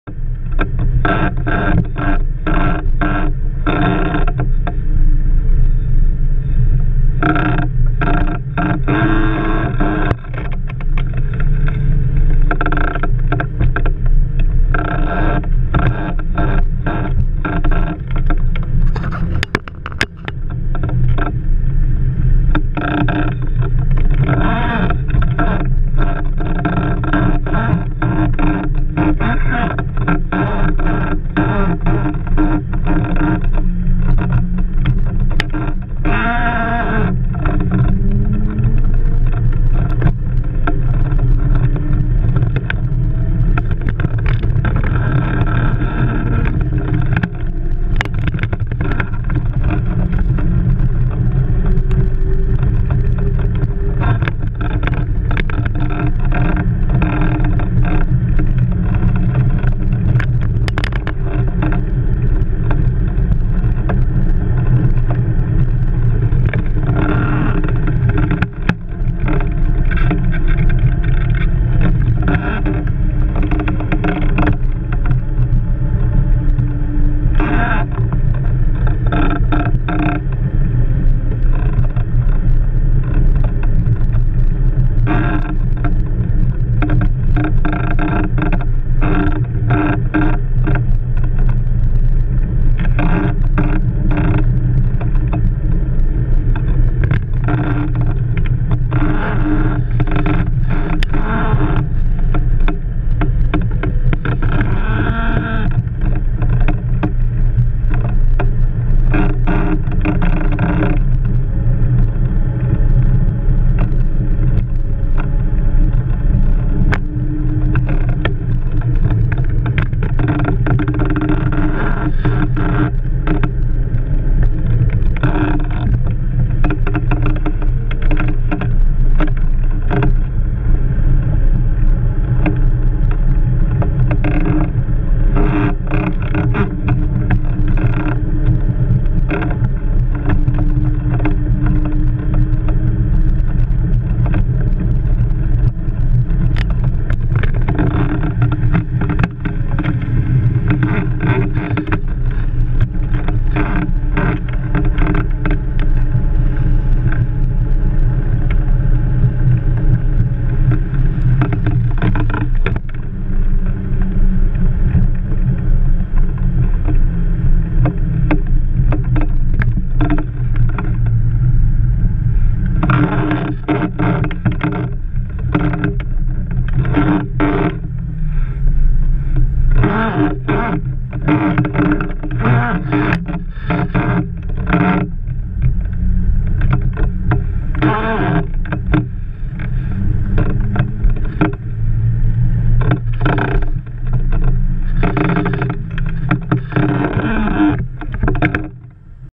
Blokarting on Ohope Beech, New Zealand!
A short video of Blokarting on Ohope Beech with GoPro Hero 3 Black Edition mounted with a GoPro Pole mount.